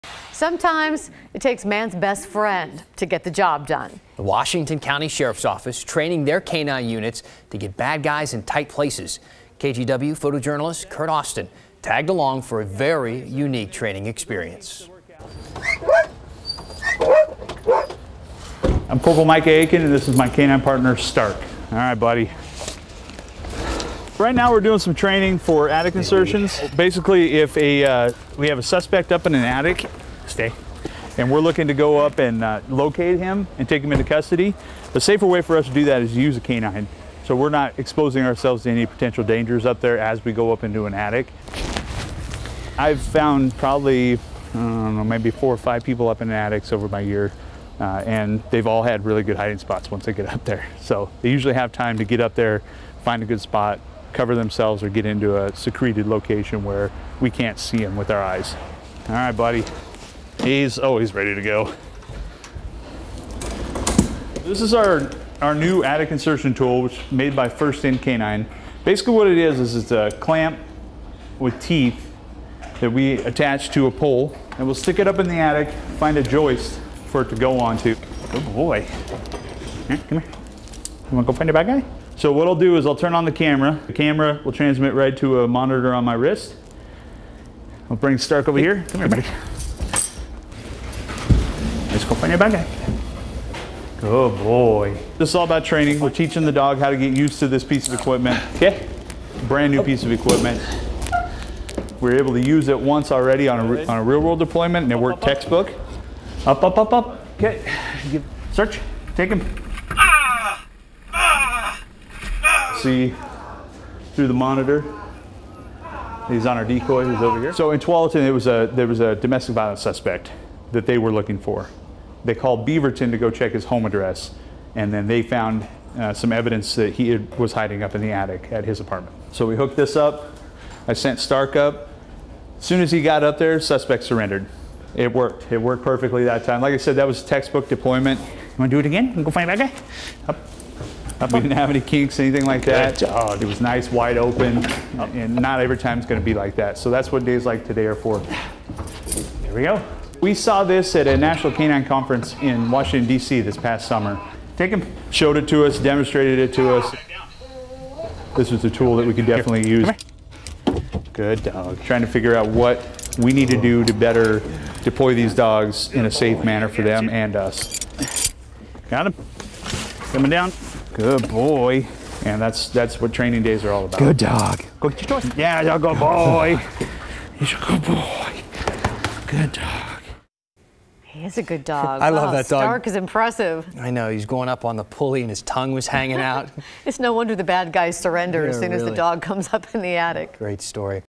Watch: K-9 'Stark' training to find bad guys hiding in attics
The Washington County Sheriff's Office shows us how K-9 'Stark' is training, with the help of a special hoist, to get into attics and find hiding suspects.